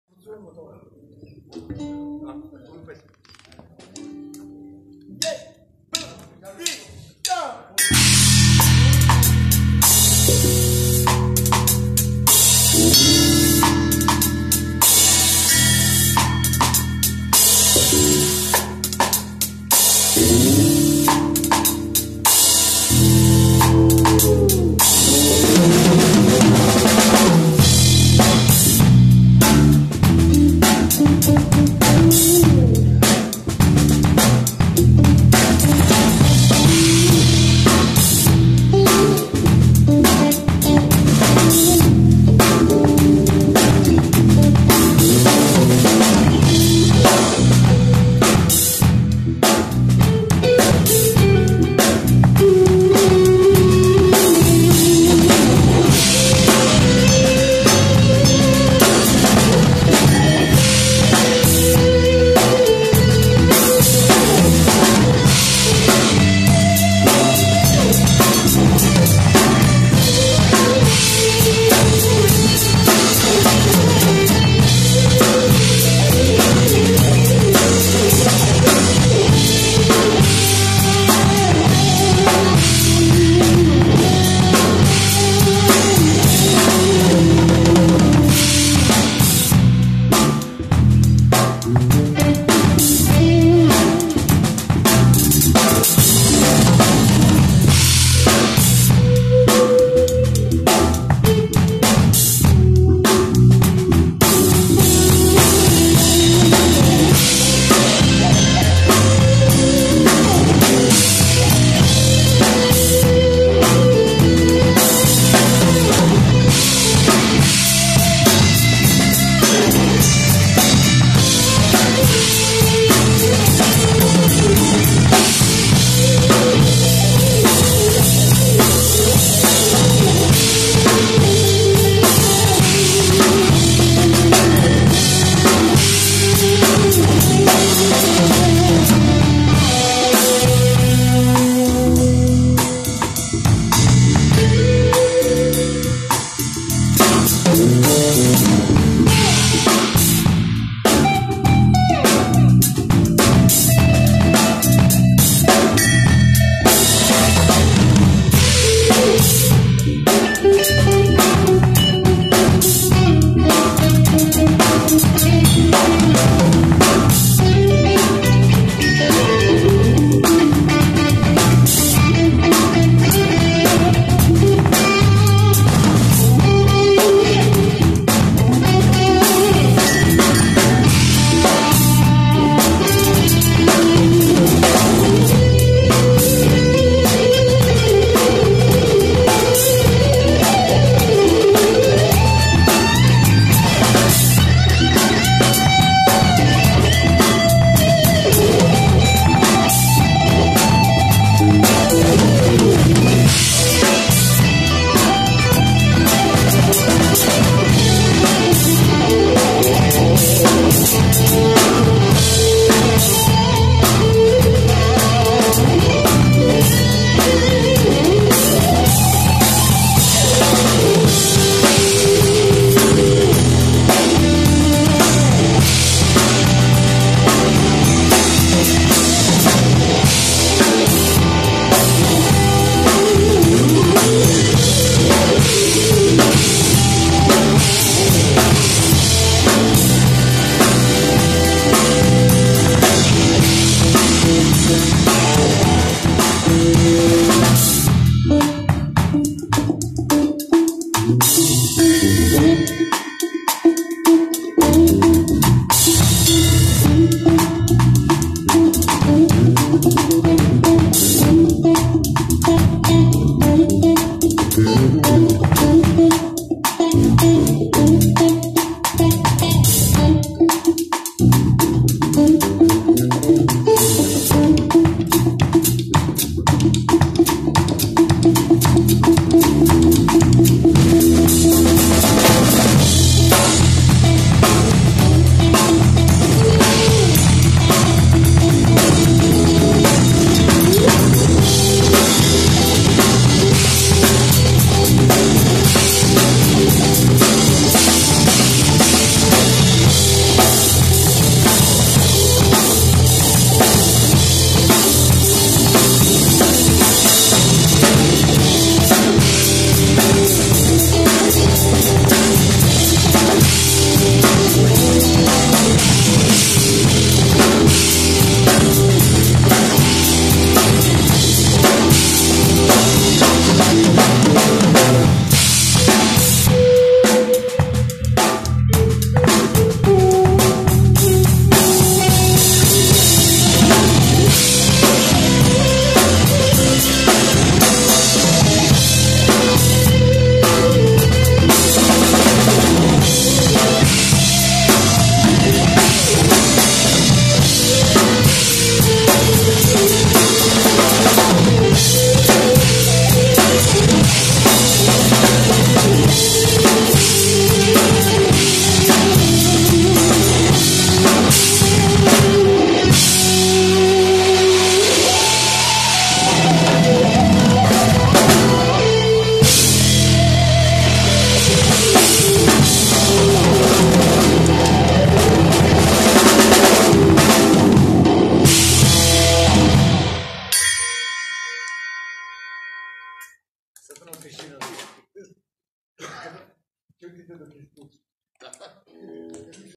Jamming Drums - Igor Velikov Vele bubanj, Bice bass, Rista gitara
Jamm sa prijateljima na poznatu temu u toku priprema za pozorišnu predstavu  " Mandragola". Snimljeno mobilnim telefonom